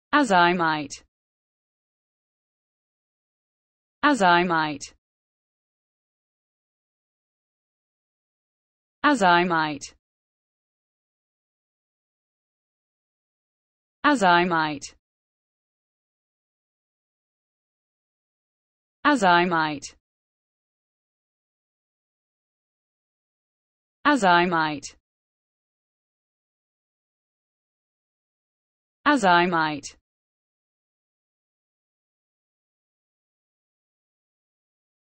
the correct pronunciation of azymite in English.
This video shows you how to pronunciation azymite in English.